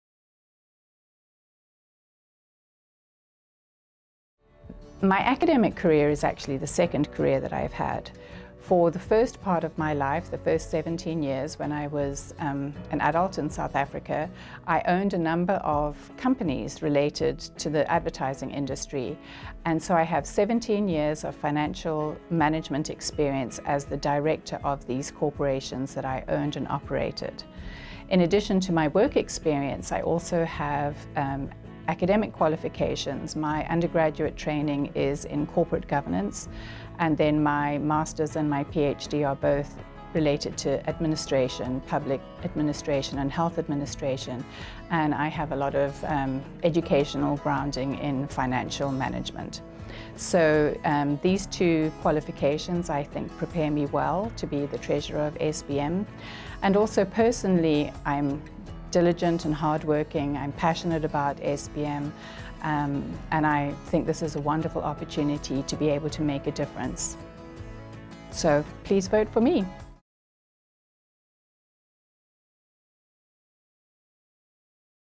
Sandra Winter: What Makes You Uniquely Qualified?
Society of Behavioral Medicine 2018 secretary/treasurer candidate Sandra J. Winter, PhD, MHA, describes her qualifications.